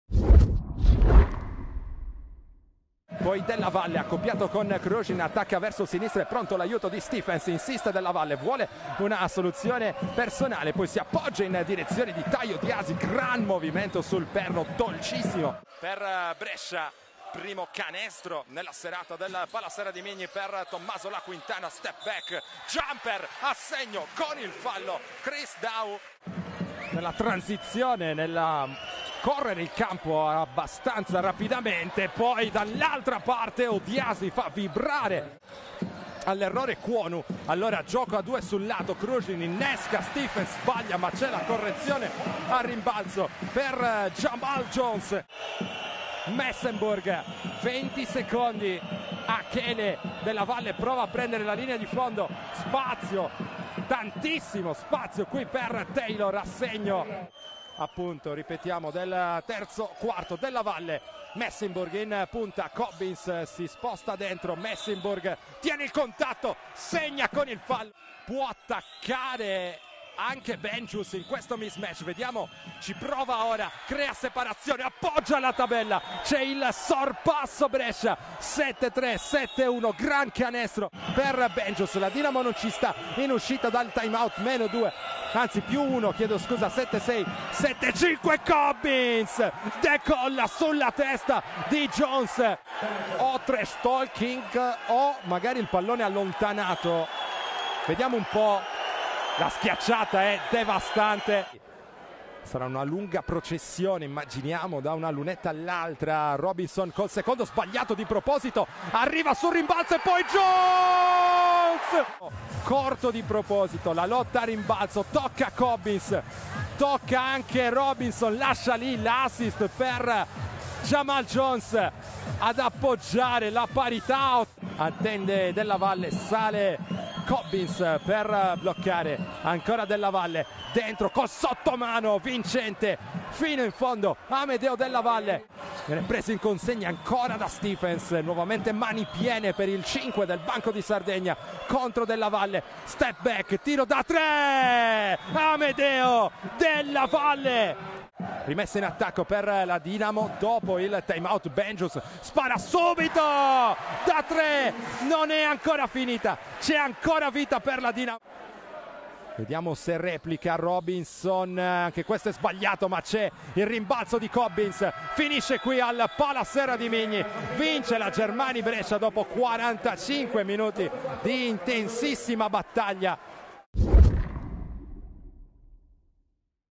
G13 LBA HIGHLIGHTS| DINAMO BDS - BRESCIA
il racconto del match tra  Dinamo e Brescia, valido come 13esima giornata del campionato LBA